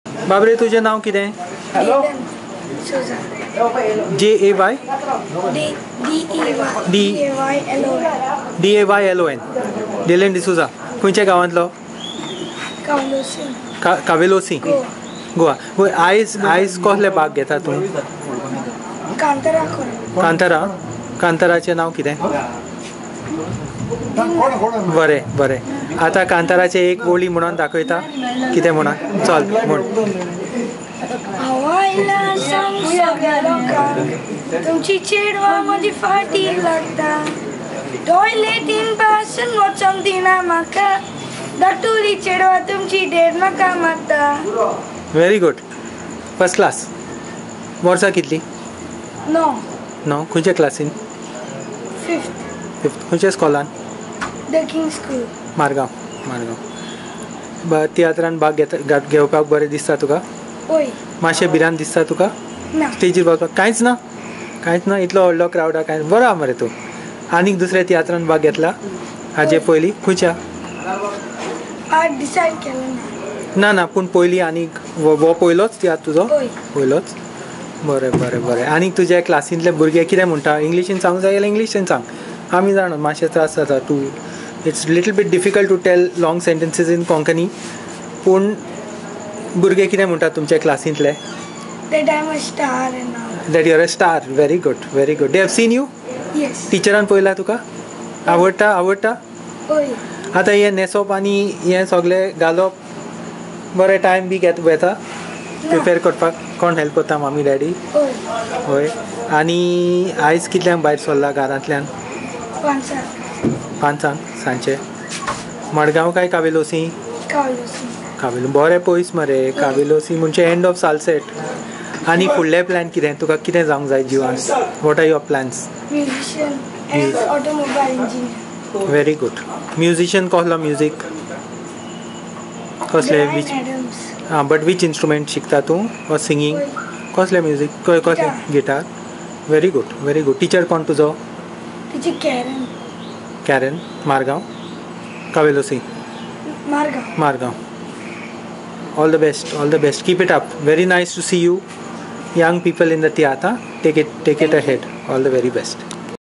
Daylon D'Souza is 9, and already singing, dancing on the Konkani tiatr stage... hear his complaint
Daylon was a bit intimidated to do this, and wanted to know: "Will it be tough?" He said he was in an English medium school... reflecting our changing times and the language loss our community is going through. His plans? Musician and automobile engineer. This Cavelossim lad is currently learning the guitar from Teacher Karen. Recorded by Frederick Noronha (FN).